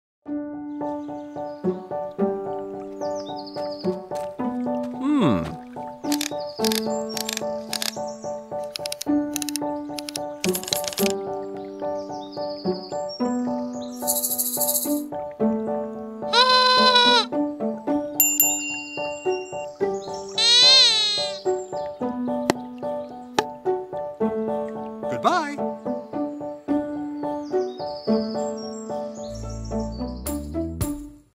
Ambi Toys in Motion - One man band
A cheery little musician with a drumstick in one hand, a double-ended whistle in the other, and a tummy drum filled with beads. His head turns and makes a gentle ratchet sound and his little hat is a bell.